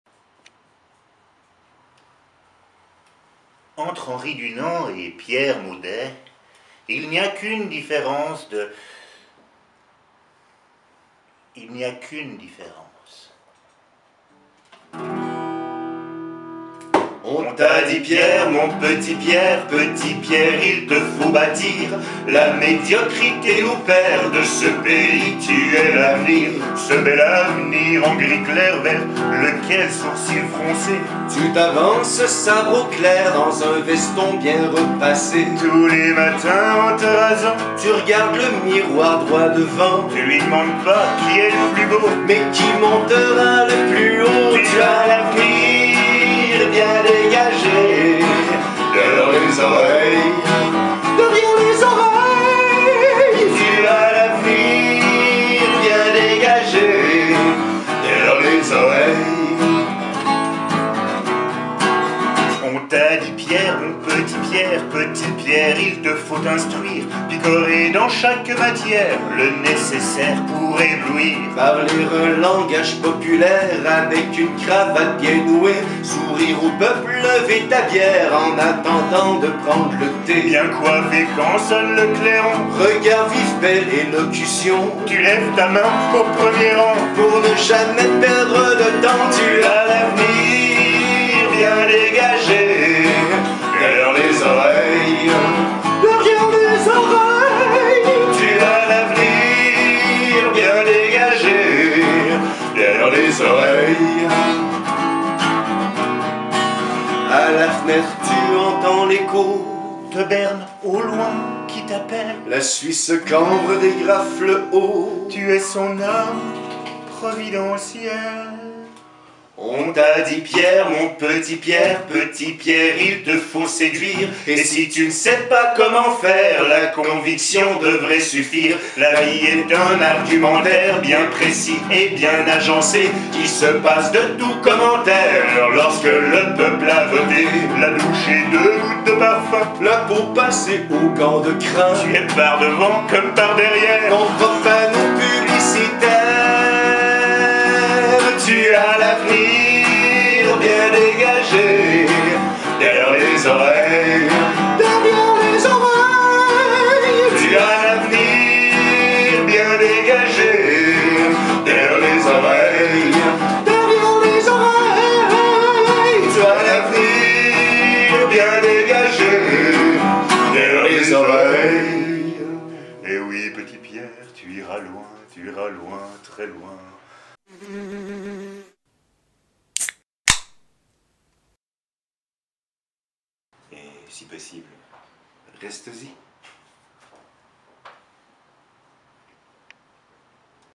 Petit Pierre Maudet
Il est notre Sarkozy calviniste, le mariage réussi du franc-parler et de la langue de bois... Adulte depuis l'enfance, mûr depuis sa prime adolescence, il est né pour être notre guide, la cravate sur un ventre ferme et plat et le clairon à la main, pour nous montrer le digne chemin. Loué soit Pierre Maudet, notre futur Conseiller fédéral à tous!